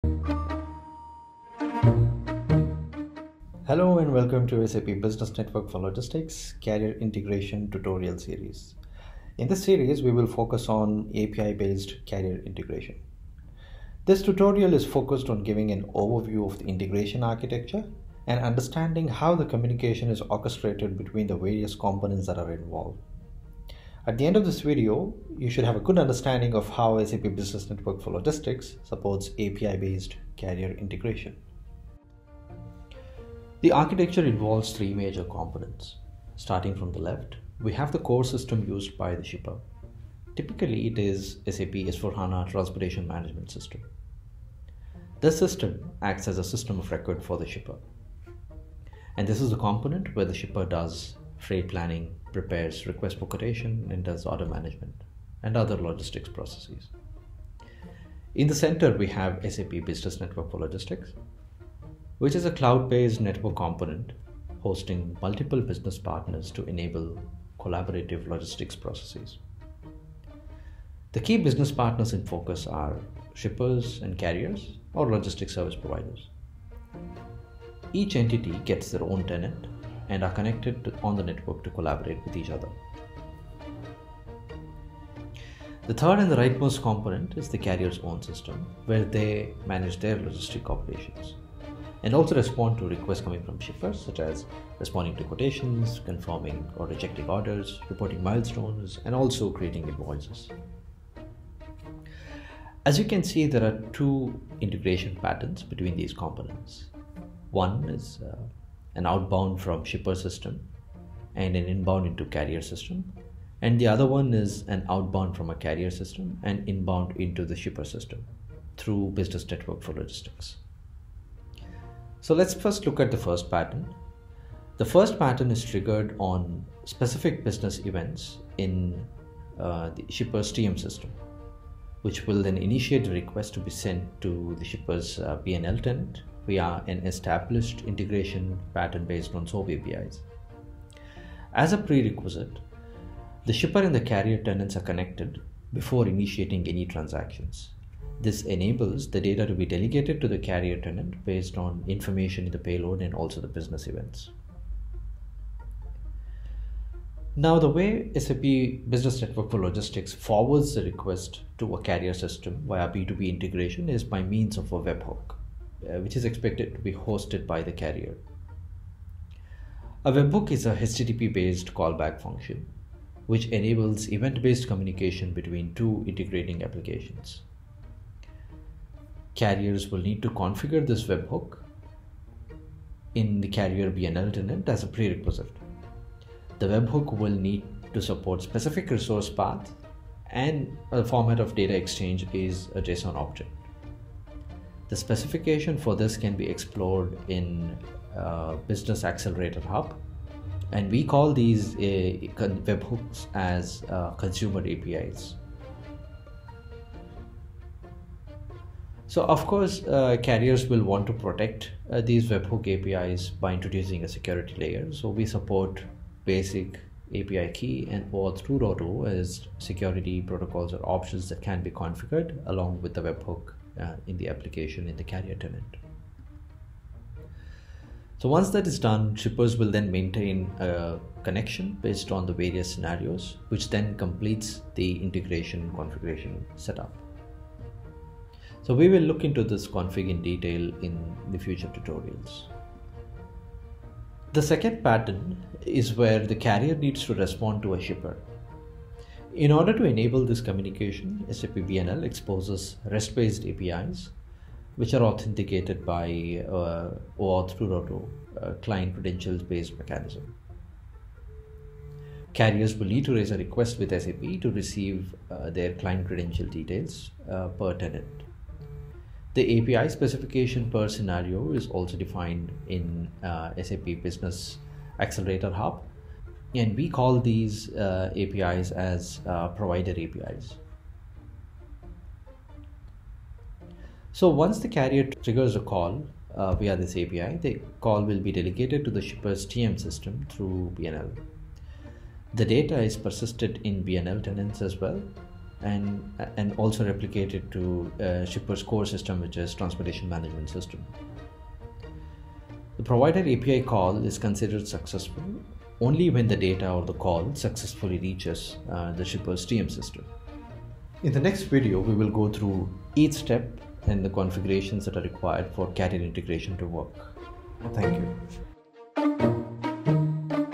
Carrier Integration via API - Overview & Architecture
In this video, you will learn how SAP Business Network for Logistics supports, API-based carrier Integration. It explains the integration pattern in detail and points out the prerequisites necessary to build the integration.